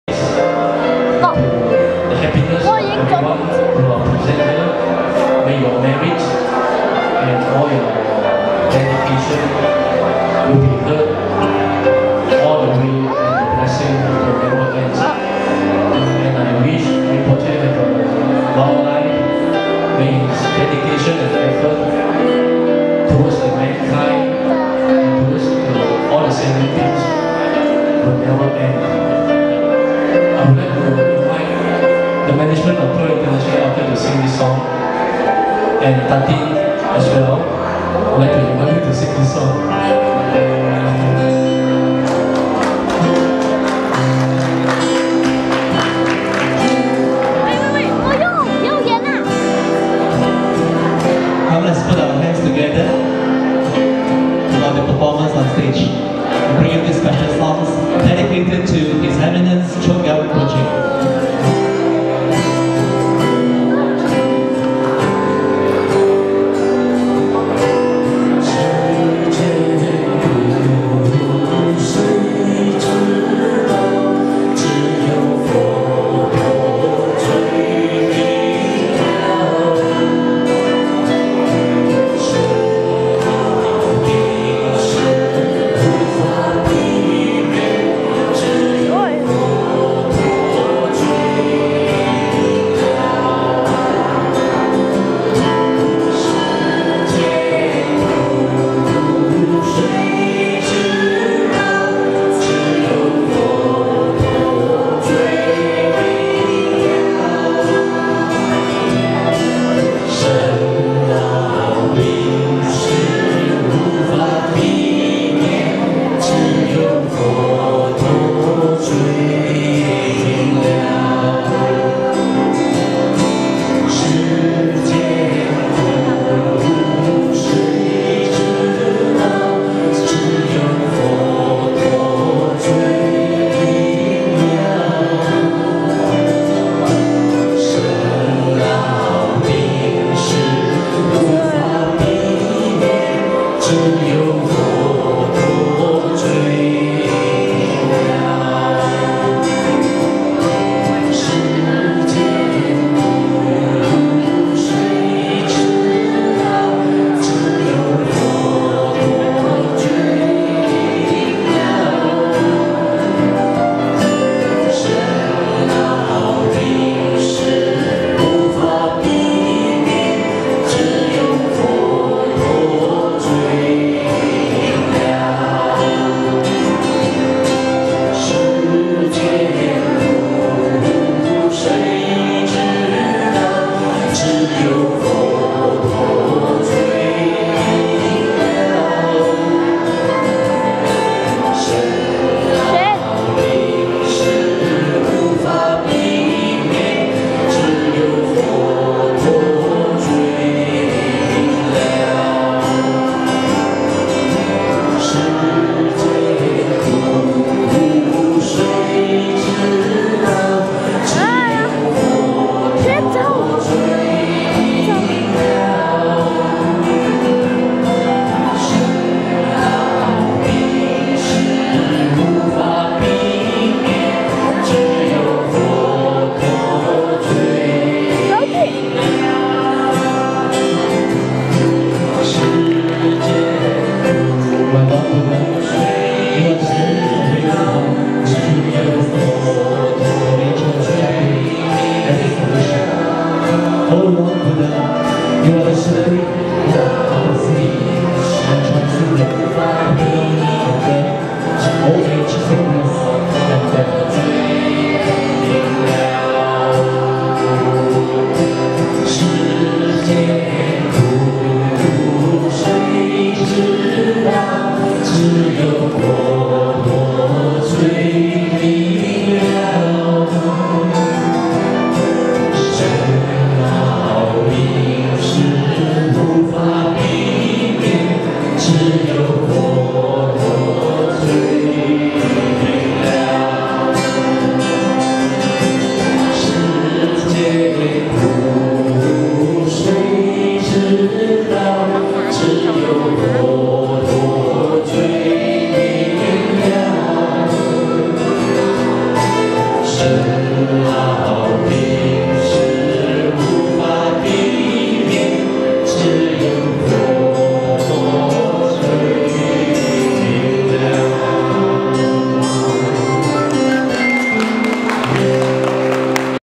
Music Offering to H.E. Choegyal RInpoche
Music Offering to Choegyal RInpoche Farewell Dinner, Music Offering by Jigmed Chojjor. May Rinpoche live a long life and continue blessing all the mother sentient being in this world.May his merit and dedication will benefits toward World peace. This event is held in Pearl International Hotel Malaysia.Cameraman  by 10 years old Lee Shen Yi.Rejoice!